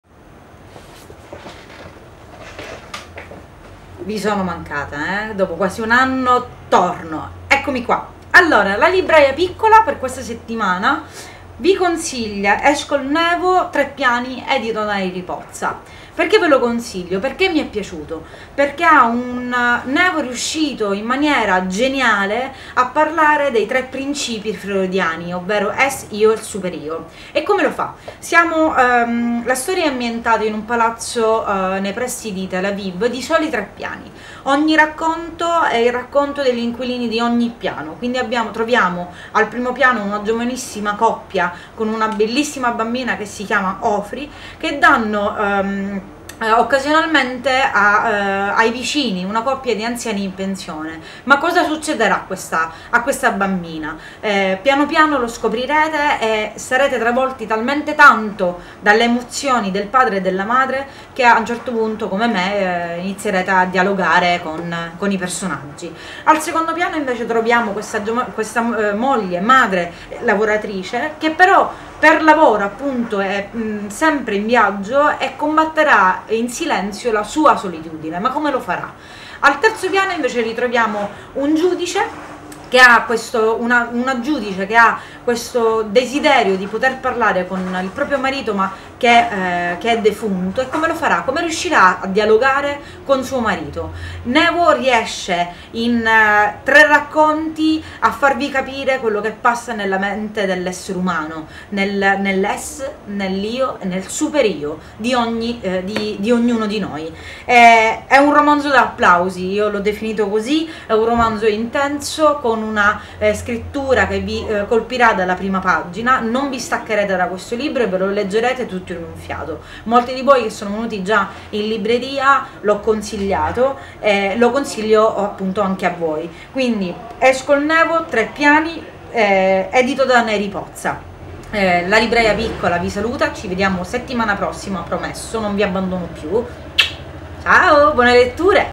IlConsiglioDelleLibraie
la libraiapiccola vi consiglia
"Tre piani" di Eshkol Nevo, NeriPozza